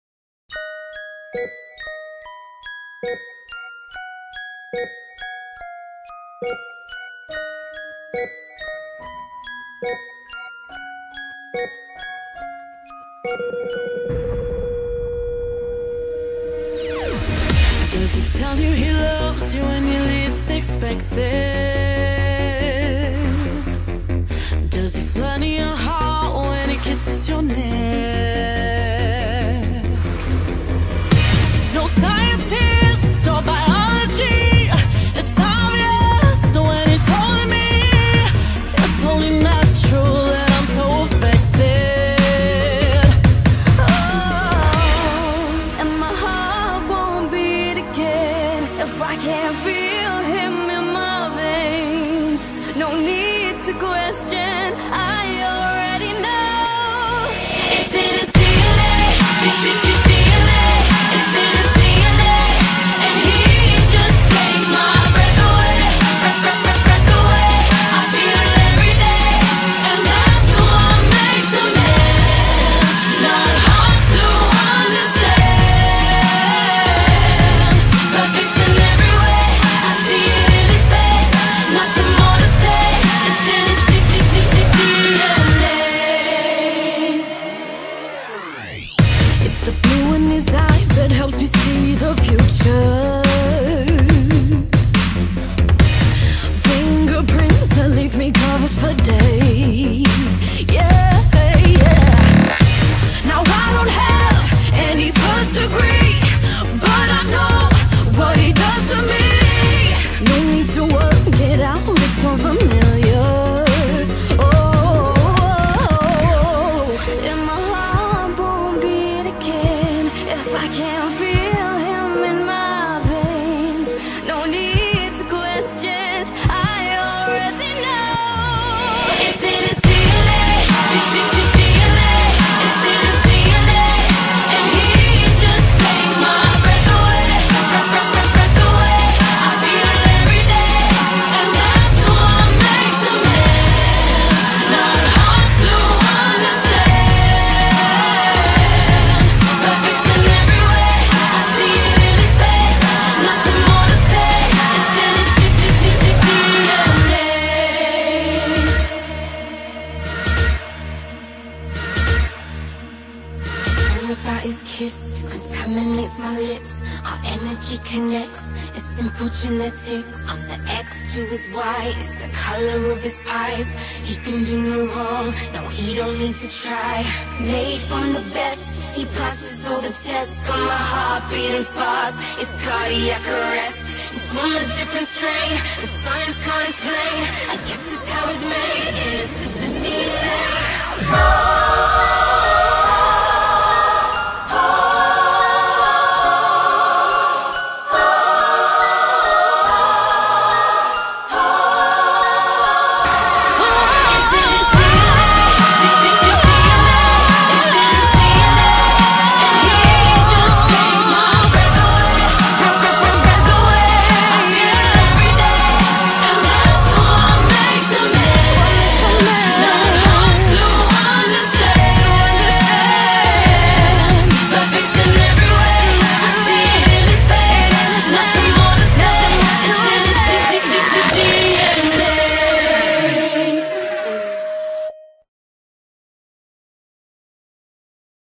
Innocence ~Trailer
Copyright goes to Little Mix and the owners of the gifs.
Quotev Trailer
Yes, I got the name Hazel and the line "She fell in love like she fell asleep, slowly than all at once" from The Fault In Our Stars by John Green.